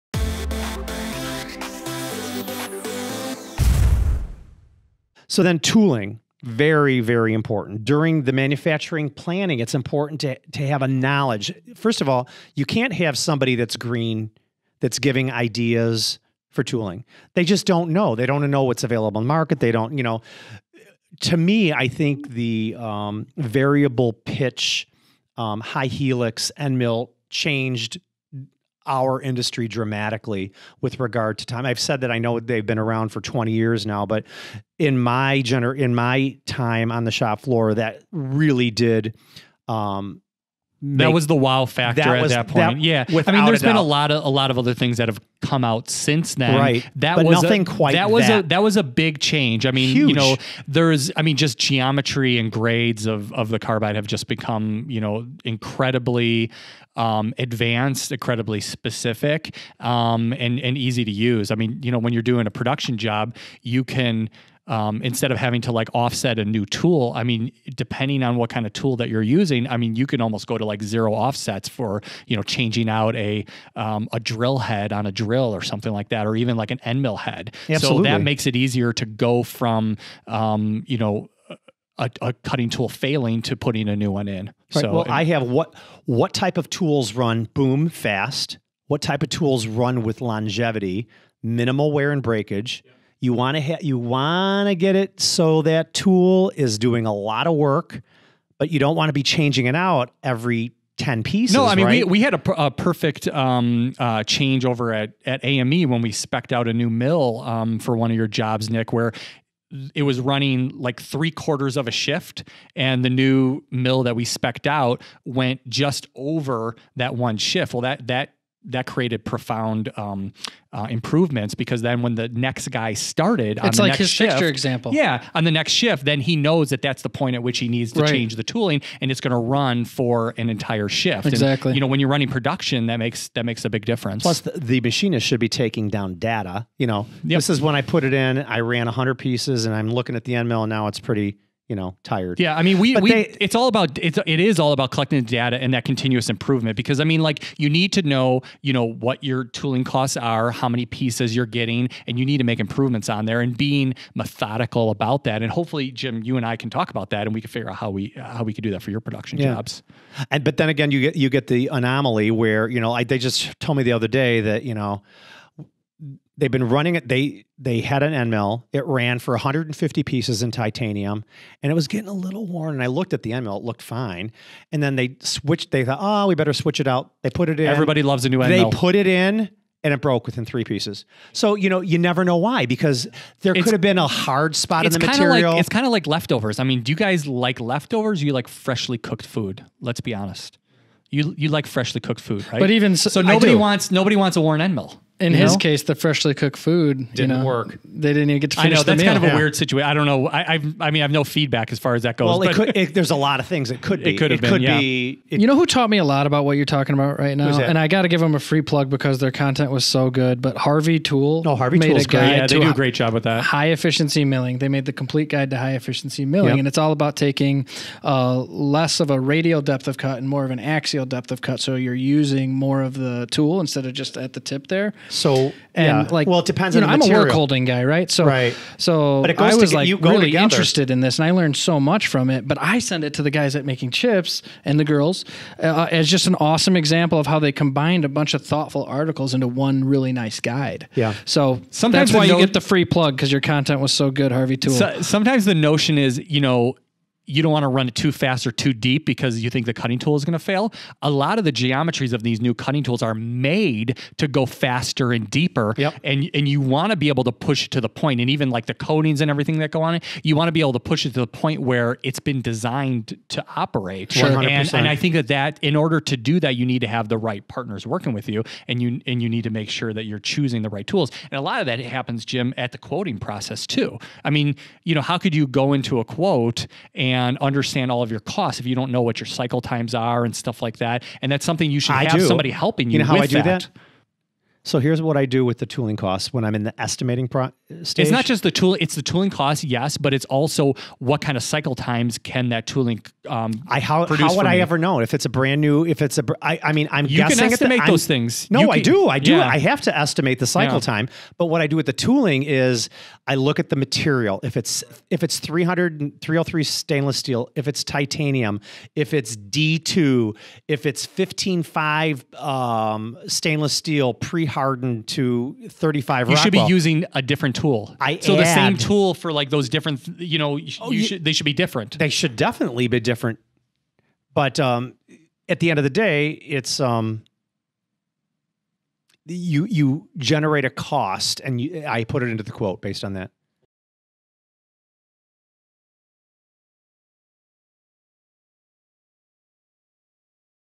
Choosing the Best Tooling For Your Production Job
In the latest episode of the MakingChips podcast, Jim, Jason, and Nick discuss the how to accommodate and properly utilize new tooling in your shop, as well as considering types of tooling before and during the quoting process.